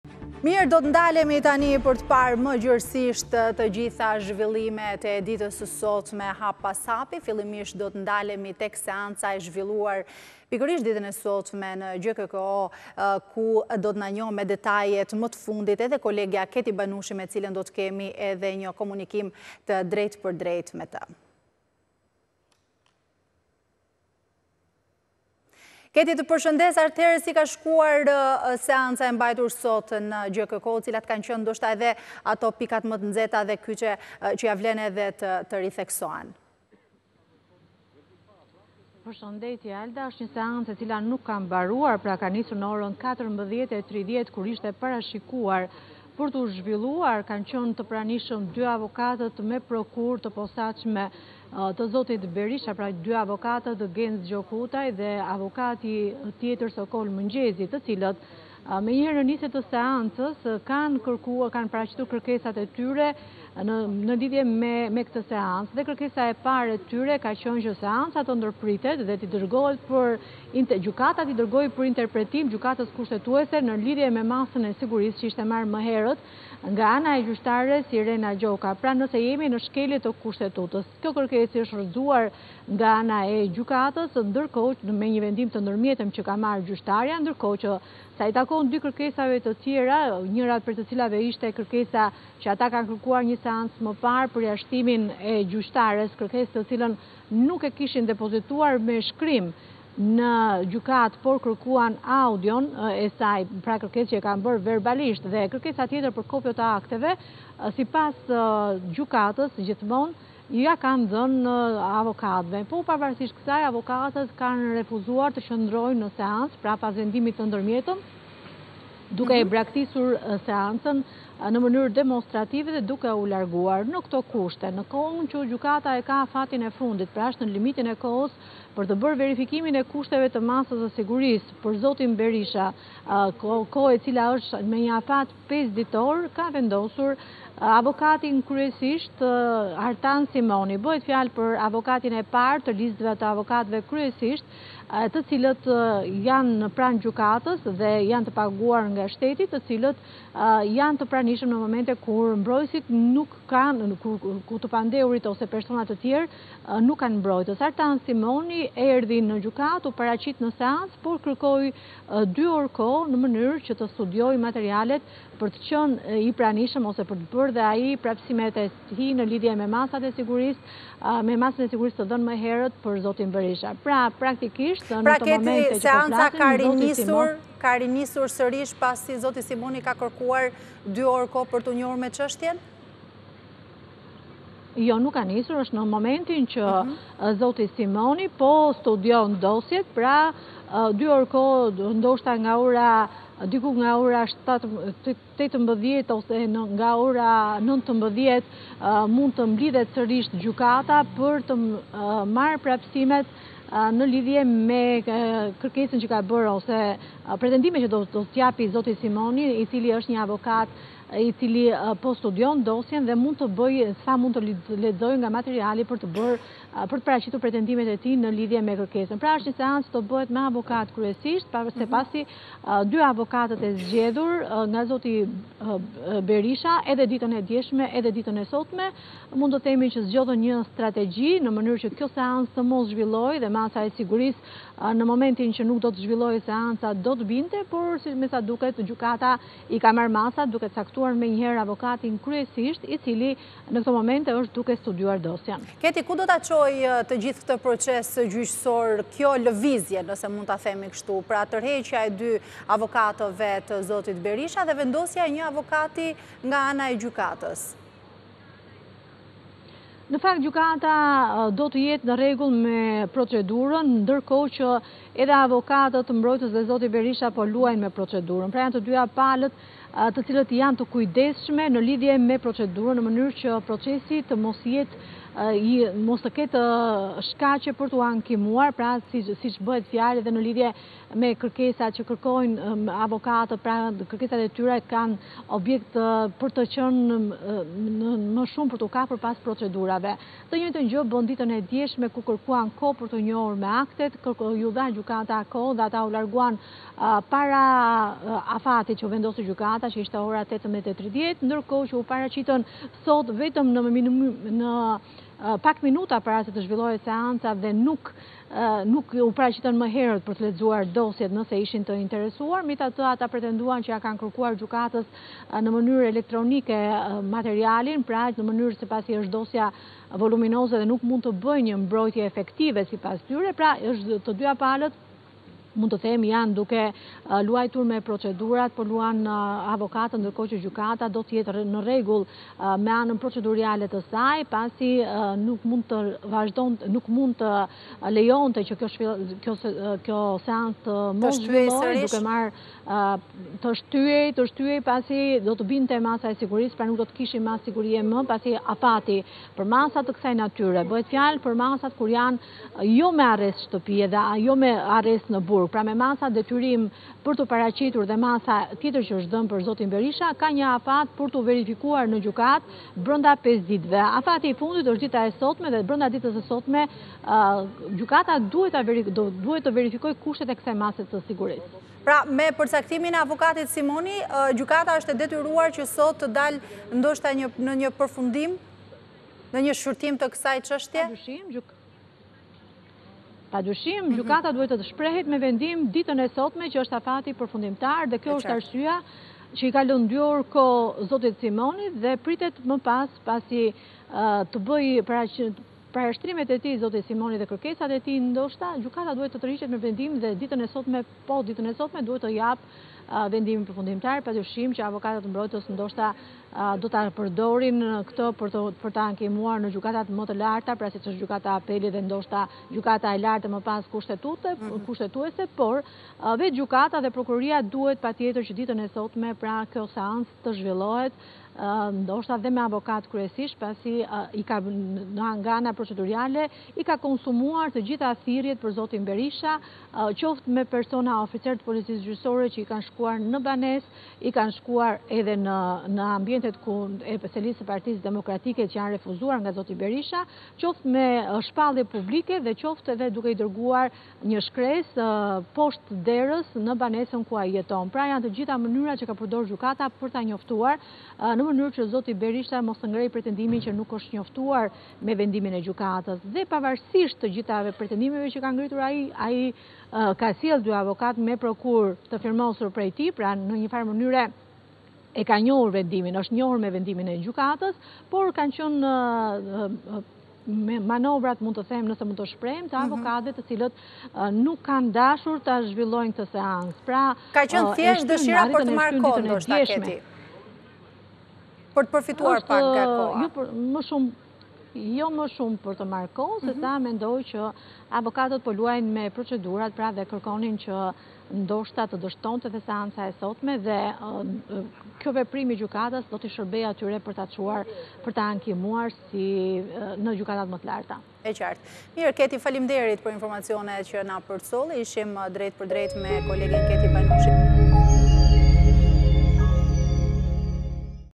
‘Partizani’,dy avokatët e Berishës braktisin seancën:U rrëzuan kërkesat për përjashtimin e gjyqtares
reportTV lajmeshqip lajmeshqip Pasi dy përfaqësuesit ligjorë të Sali Berishës kanë braktisur seancën për aferën e Partizanit, GJKKO ka vendosur të caktojë një avokat të paguar nga shteti për të vijuar procedurën. Bëhet fjalë për avokatin Artan Simoni i cili kërkoi 2 orë kohë për t'u njohur me aktet.